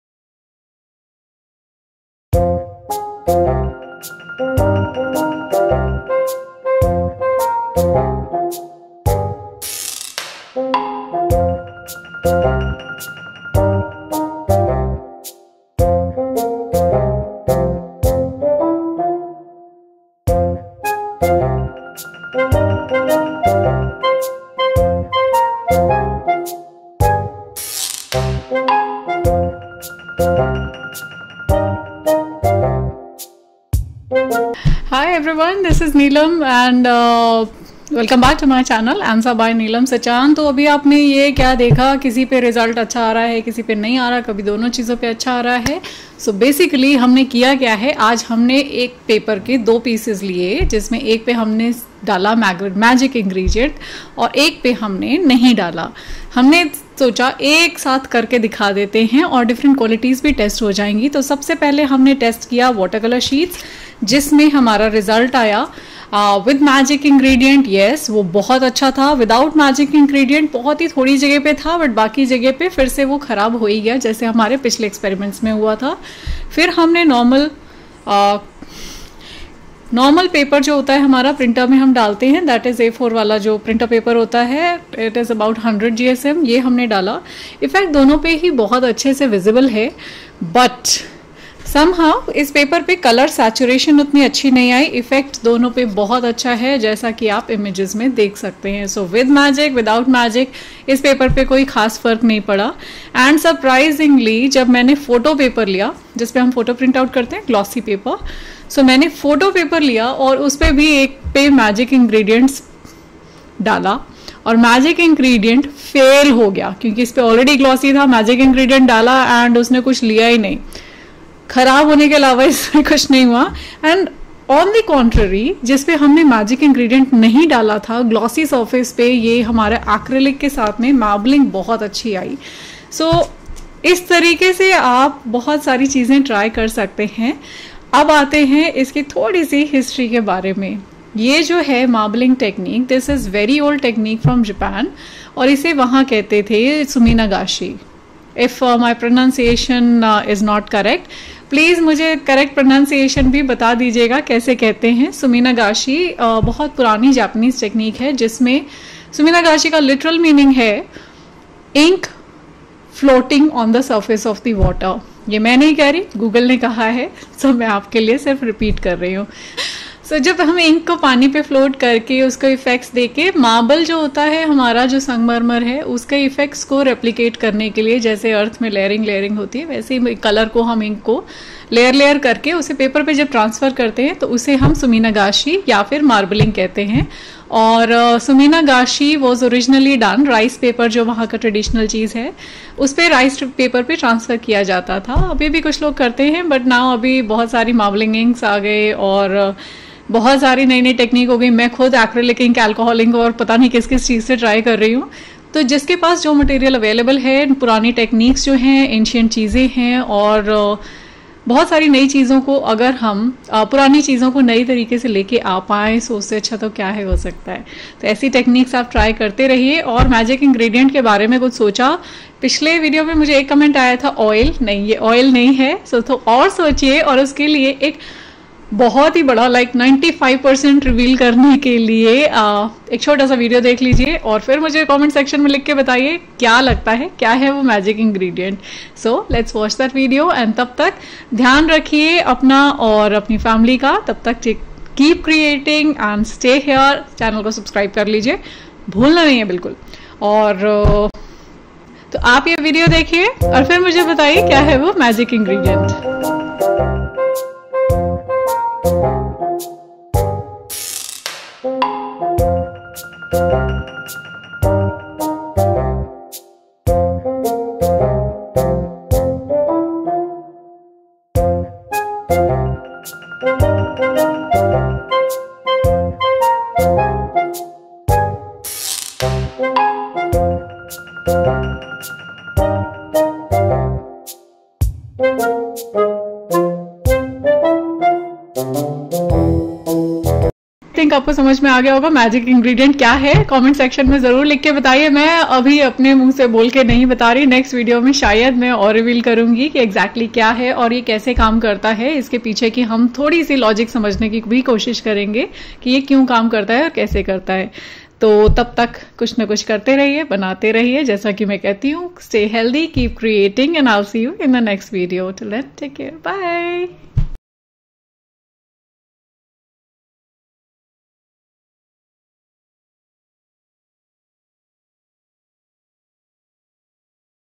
TESTING SUMINAGASHI on various paper qualities with magic ingredient - BIG REVEAL
the paper qualities used in this video are :
normal 100 gsm printer paper
photo glossy paper
watercolour sheet 200 gsm
acrylic paints for marbelling or suminagashi